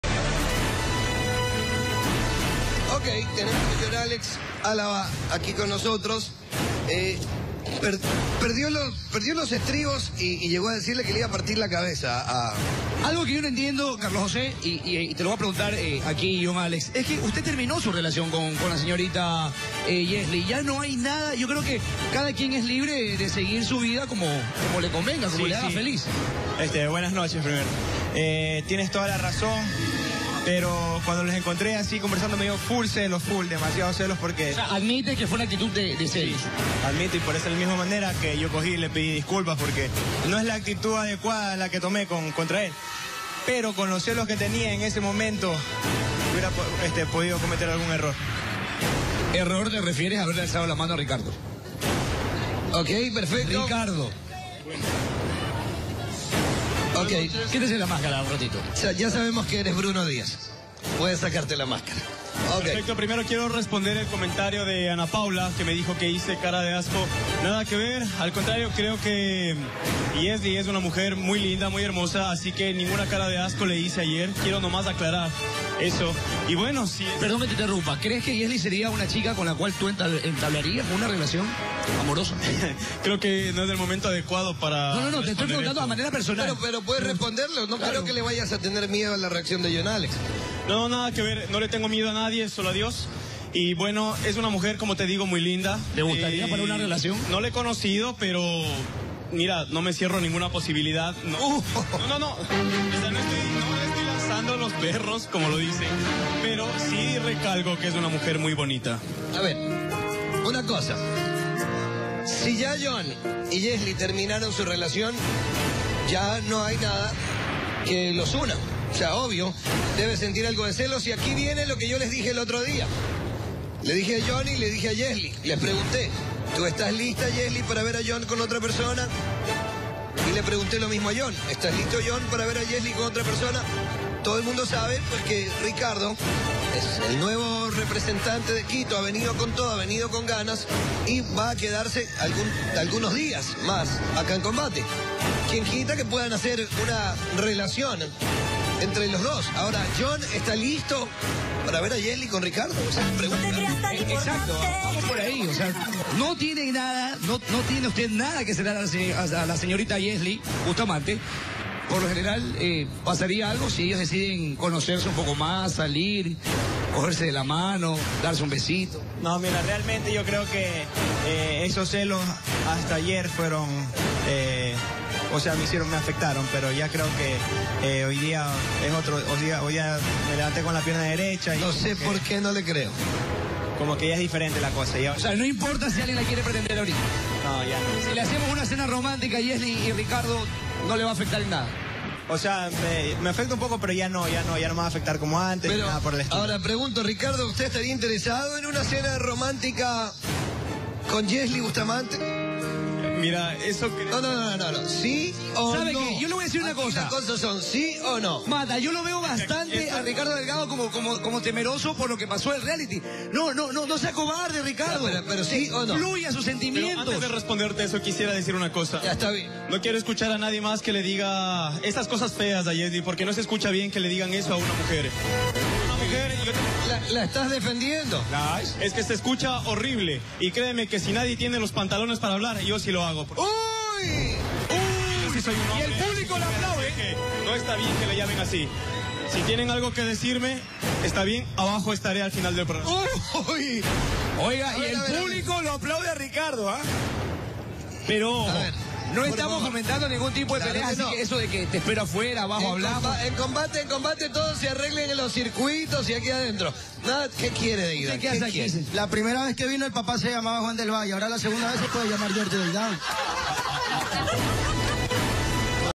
Jhon Alex celoso de Ricardo
Ricardo y Yesli están cada vez más juntos y esta situación le incomoda al concursante.